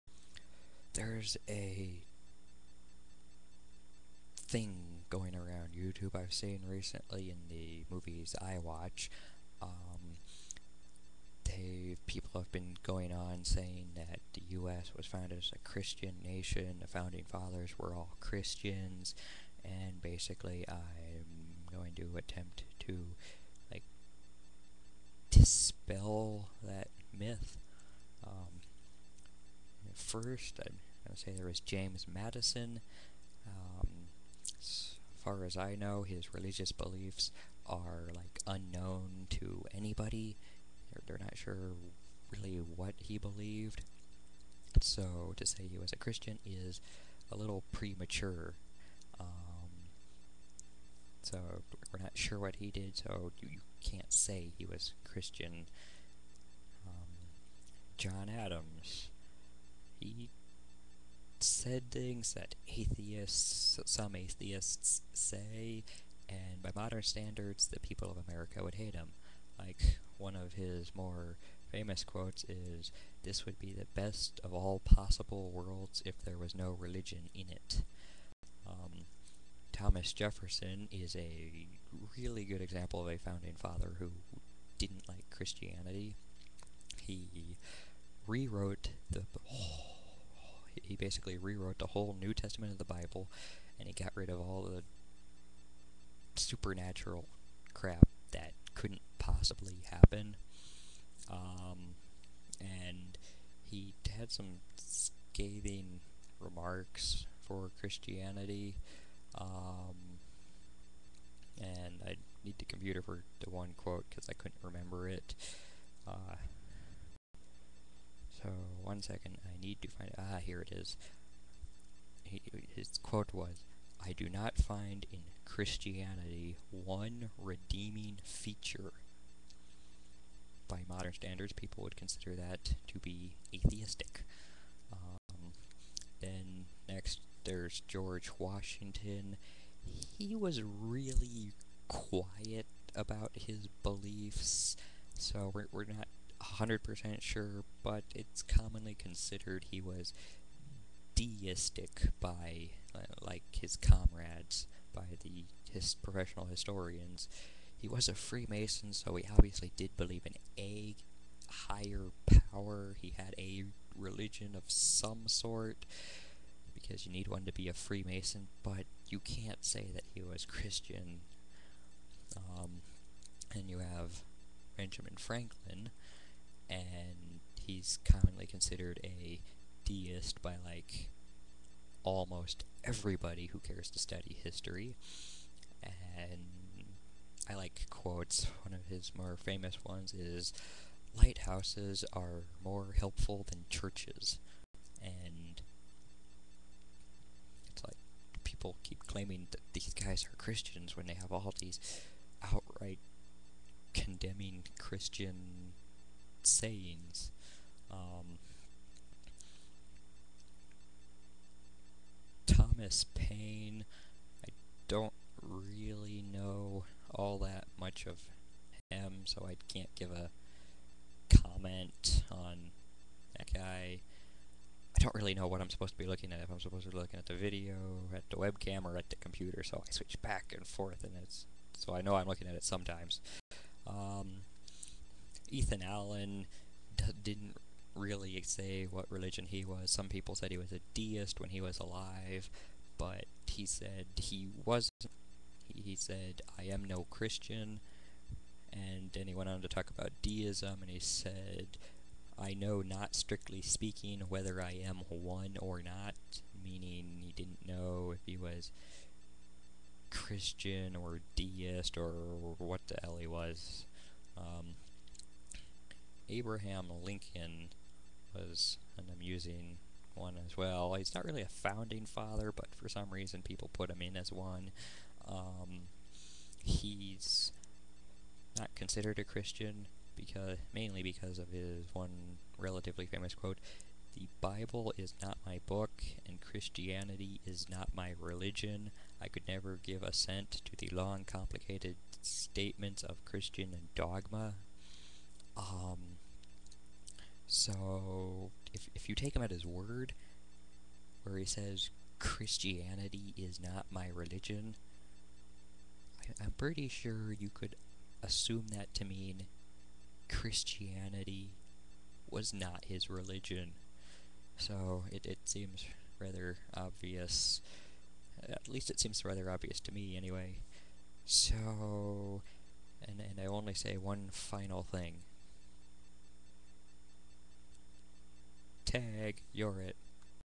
The founding fathers were not Christian, at least not by todays standards.
I have heard about people saying America was founded a Christian nation. That is not true. I think I may have forgot my side story about the pilgrims coming here as christians so that counts, but it doesn't as the Native Americans came here first and they weren't Christian.